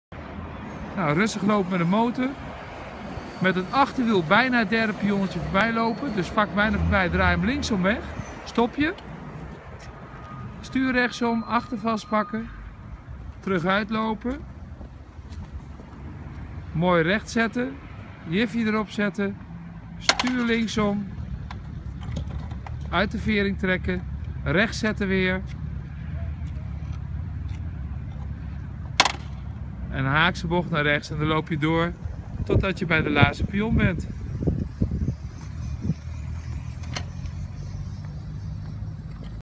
Verkeersschool Weber: de motor parkeren in een parkeervak
Lopen met de motor en parkeren in een denkbeeldig parkeervak